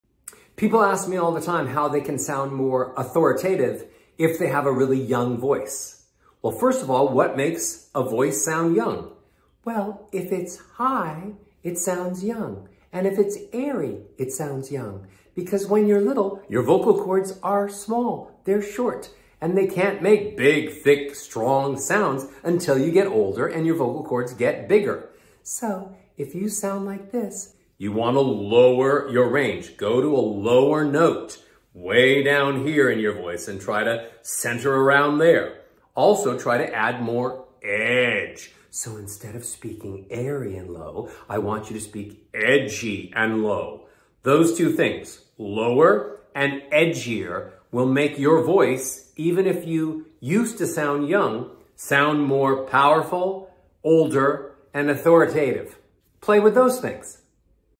How to Sound More Authoritative
This is a question I get asked a lot! If you're frustrated because your voice sounds young, this is for you.